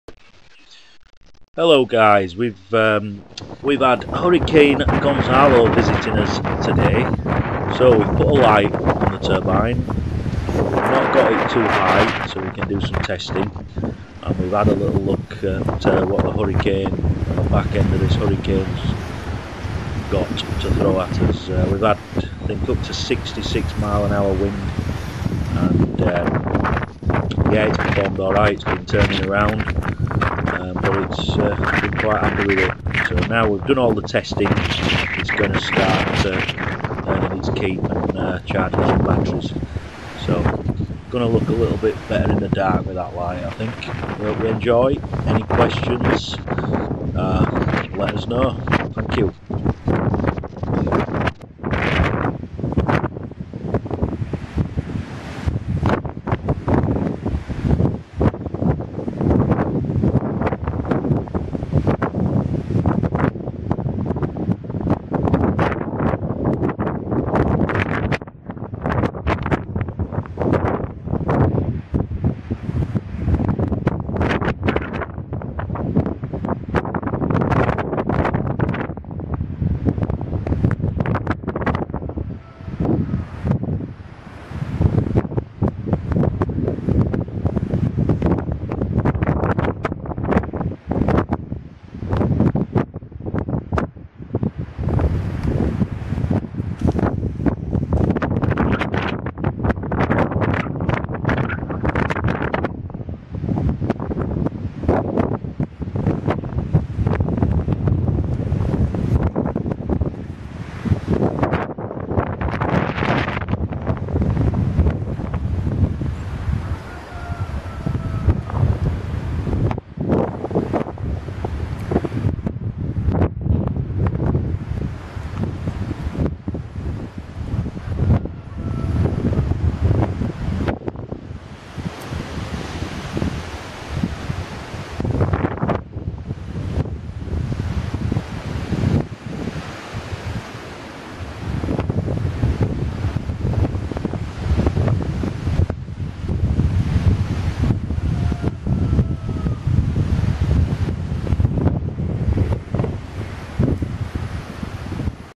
HURRICANE GONZALO VISITS WILLIAMSWINDTECH
WE GOT THE BACK END OF THE HURRICANE TODAY SO TIME TO DO SOME TESTING, - FREE WIND, PROVIDING FREE ENERGY, PROVIDING FREE LIGHT THIS WIND CAN BE DAMAGING AND ITS A WARM UP FOR WHAT THE WINTER IS GOING TO BRING.